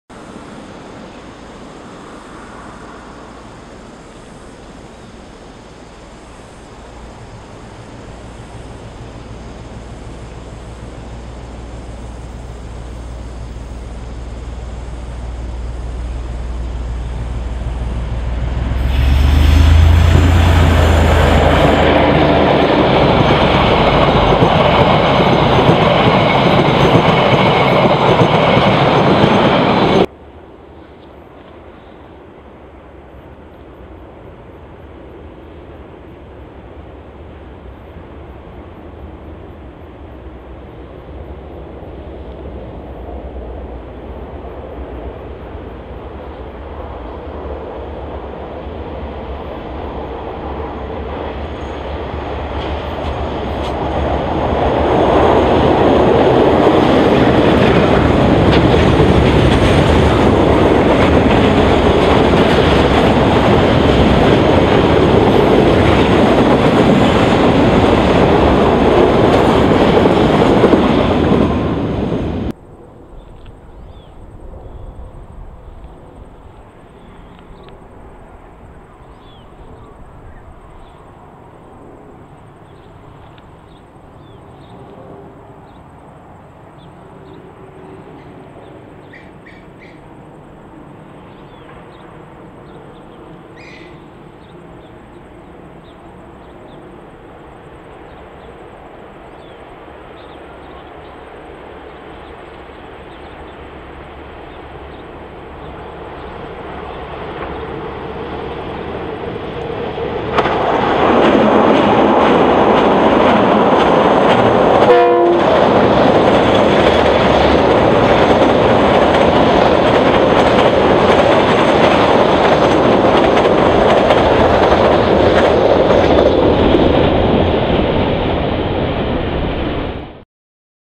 Kiwirail Main North Line Trains.
Trains on Main North Line 26/02/2012.
0:00-0:29 Tunnel 9 MNL from SH1.
0:30-1:11 Approaching tunnel under SH1 near Hawkswood Road.
1:16-2:10 Bridge 55 MNL near Baxters Road.

Feel free to hit the 'like' button. Thanks!